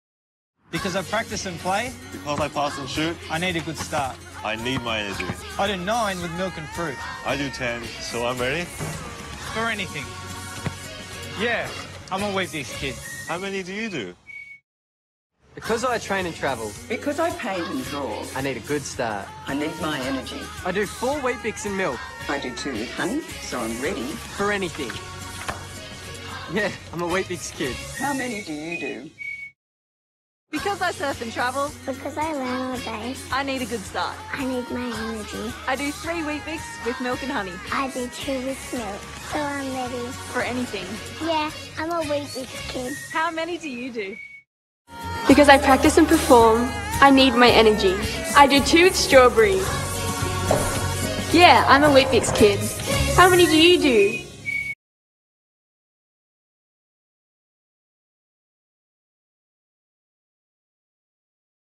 Weet Bix - Tim Cahill, Steve Smith & Steph Gilmore TV Commercial 2017
Weet-Bix - Tim Cahill, Steve Smith & Steph Gilmore Australian TV Commercial 2017

Tim Cahill does 9 Weet-Bix with milk and fruit and Emeke does 10.

Steve Smith does four Weet-Bix in milk and Gaye does two with honey. Steph

Gilmore does three Weet-Bix with milk and honey and Alyssa does two!

How many do you do?